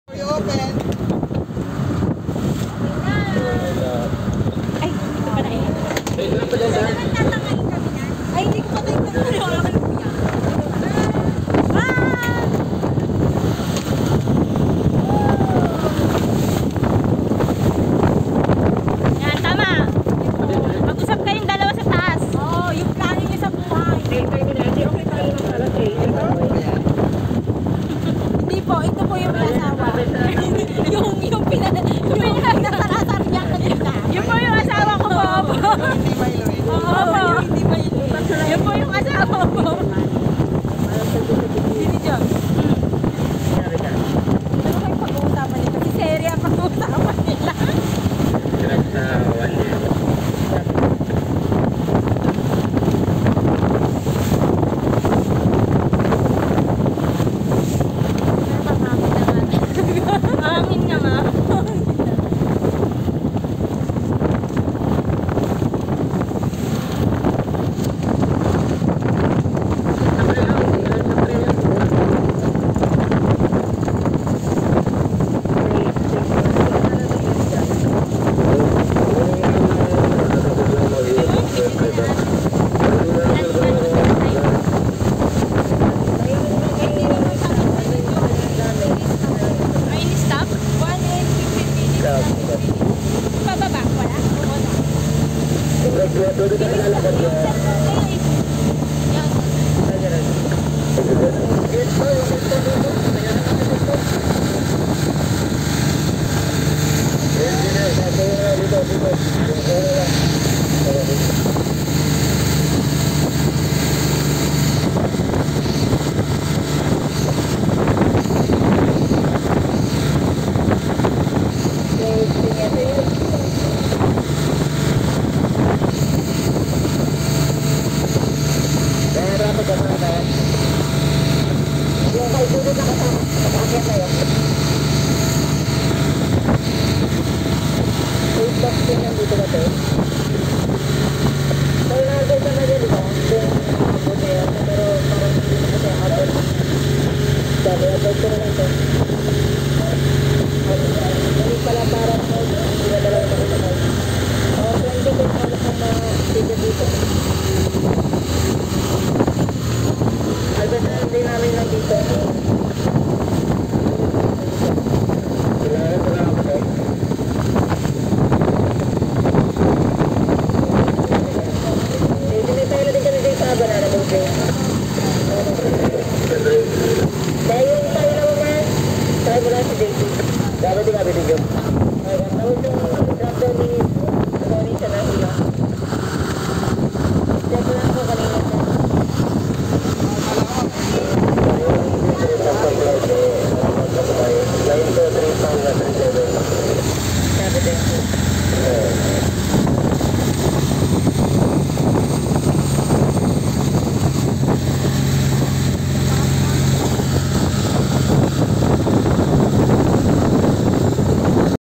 Boracay Parasailing 🏝️🏖️🌅
Our first ever Parasailing Experience in Boracay! 📣🥳🤗